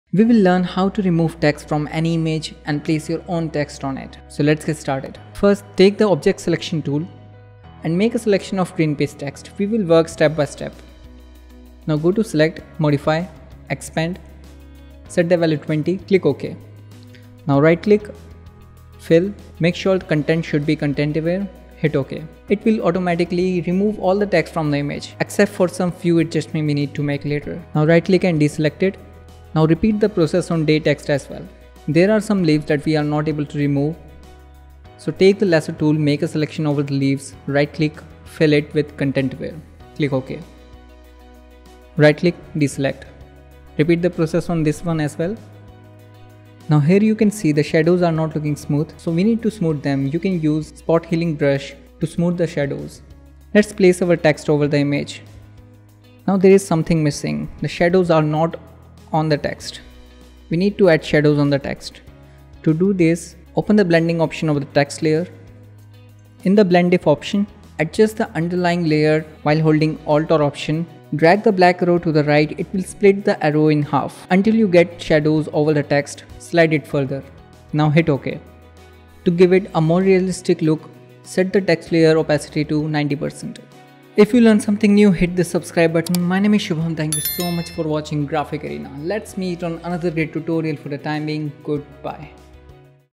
How to Remove text from image
Want to learn how to remove text from image? In this video we will learn the techniques and issue related to text removal from the photo in photoshop.